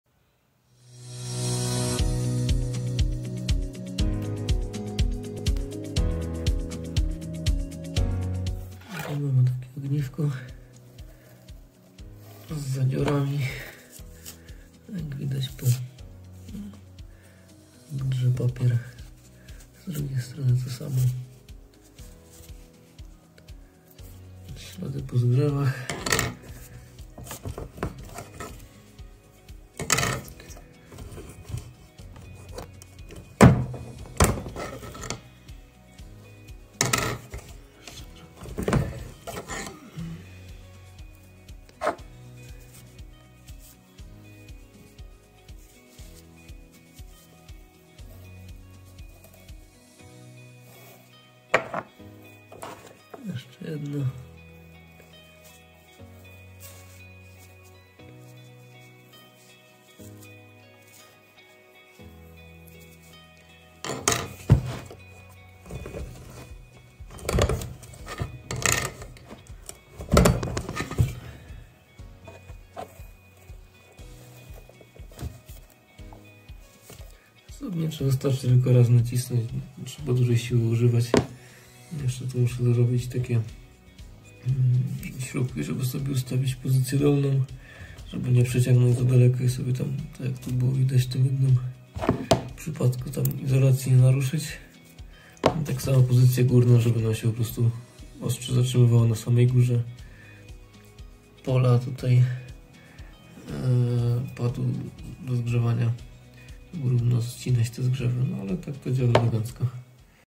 Urządzenie do oczyszczania terminali z pozostalosci zgrzewów ogniw depakietowanych 18650 I 21700
Urządzenia są w sprzedaży
Jest dodatkowa możliwość dokupienia adaptera umożliwiającego korzystanie z gilotynki jak z zwykłej praski

Więcej projektów na:
Akucad.com